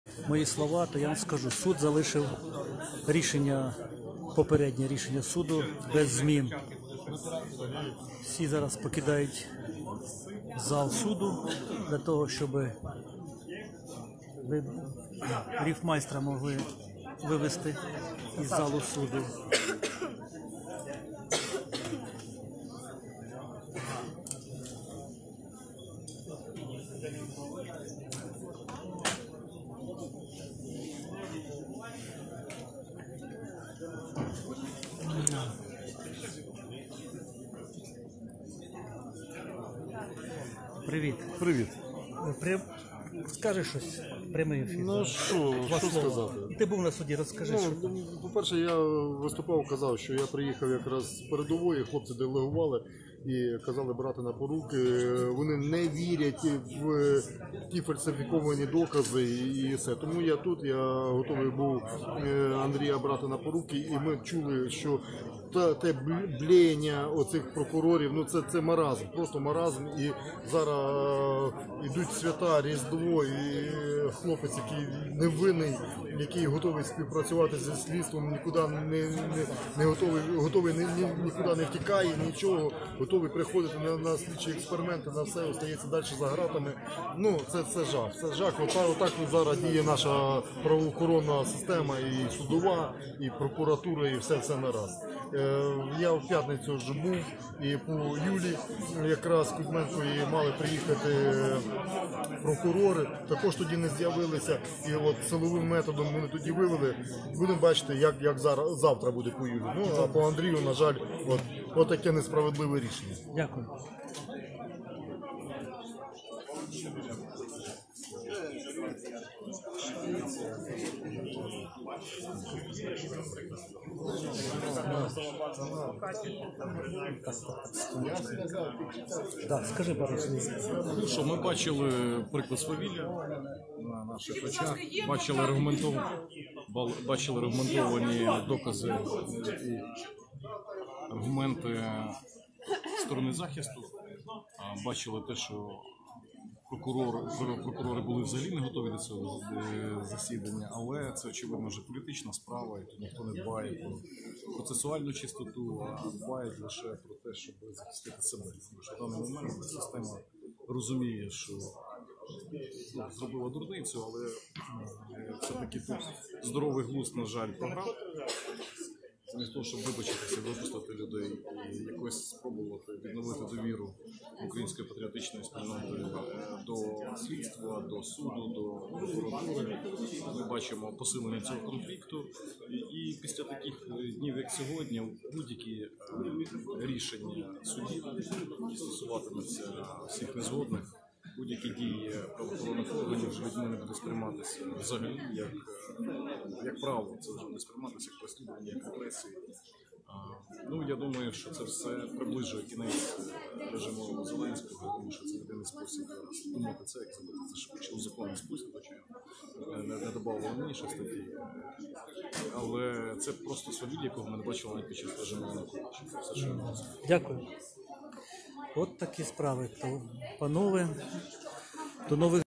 Апеляційний суд по справі Ріфмайстра залишив його під арештом
Влада остаточно підтвердила політичну причину арешту підозрюваного у справі Шеремета